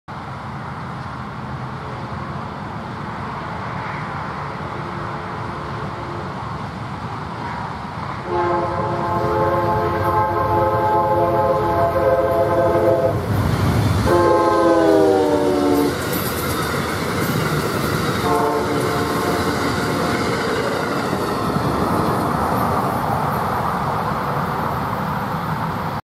Amtrak P091-10 Silver Star With Quilling K5LA Hybrid By Boynton Canal 8-11-19
Here we see SB Amtrak P091-10 with a nice K5LA hybrid as it flies through near Boynton Canal.

Power:

AMTK 172